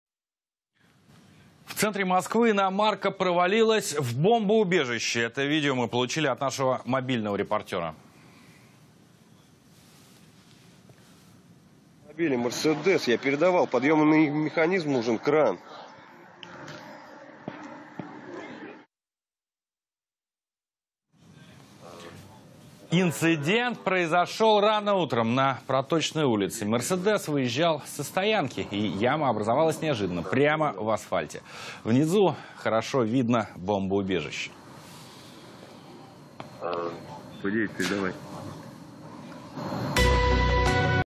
В центре Москвы иномарка провалилась в яму
Под провалом в центре Москвы оказалось бомбоубежище